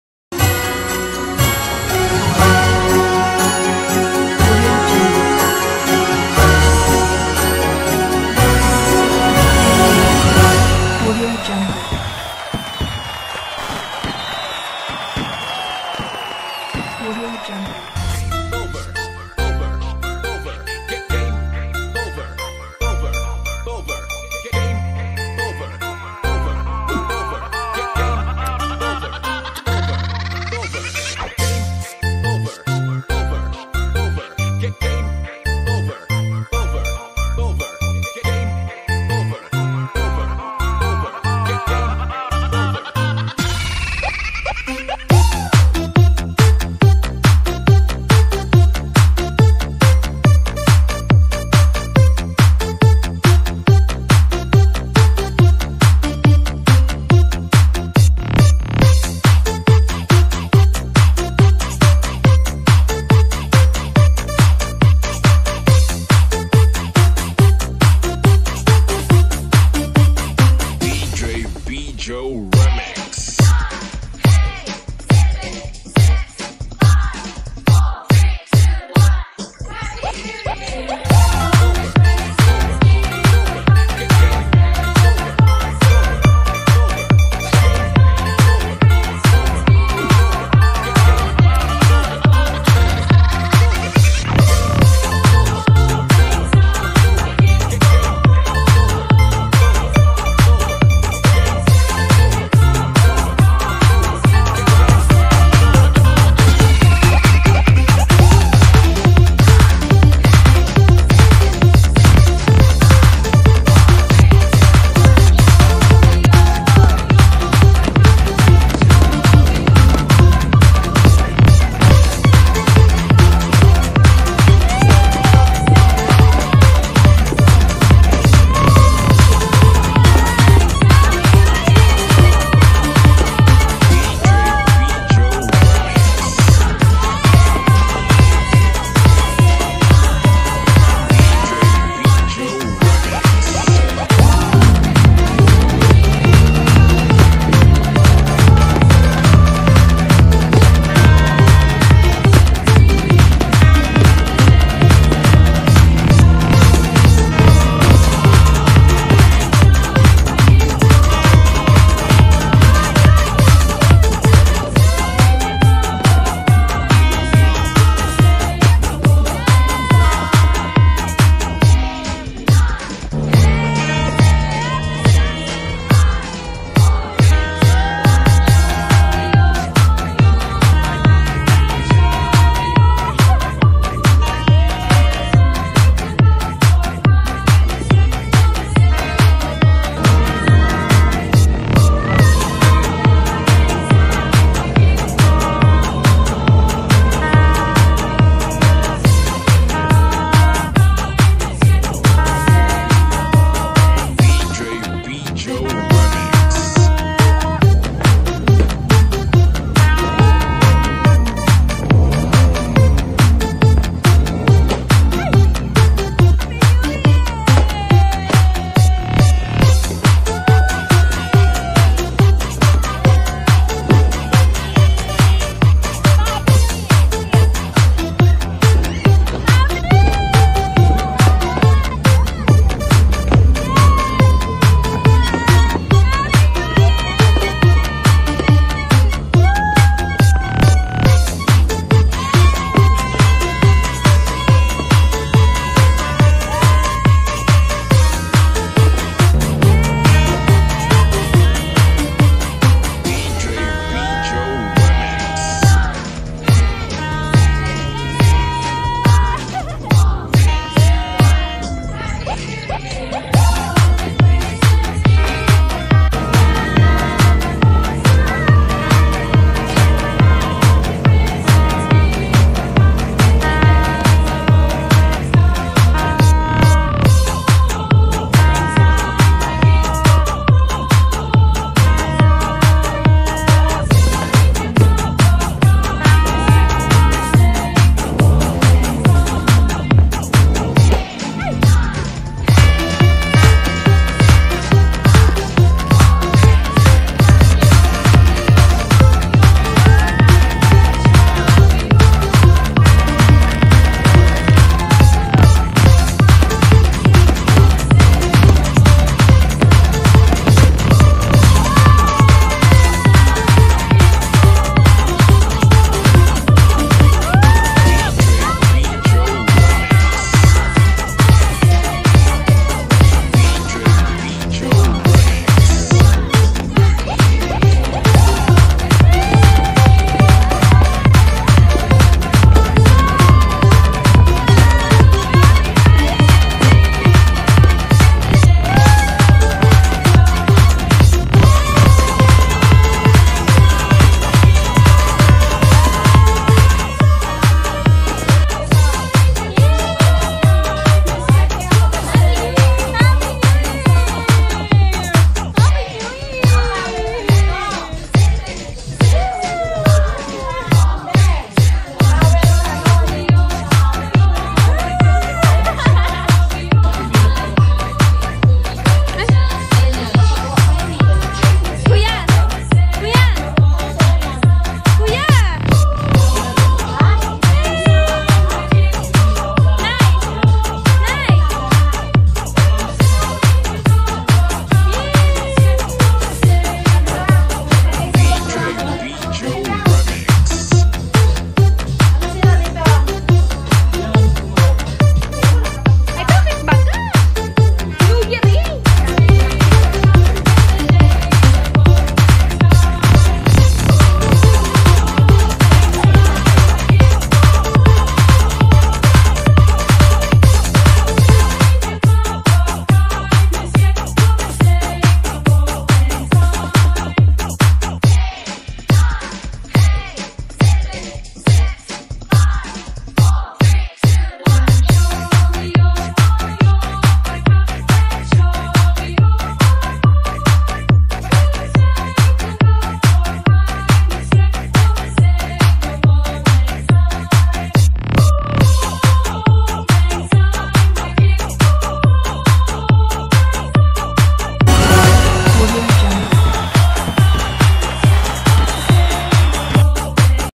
Happy New Year - Have a blessed 2022 (Fambam) | Kira Vizcarra
Thankyou Lord for Everything! Have a blast everyone. Thankyou for the complete and blessed family.

More cheers years to come.✨
Welcome 2022. ❤️🤞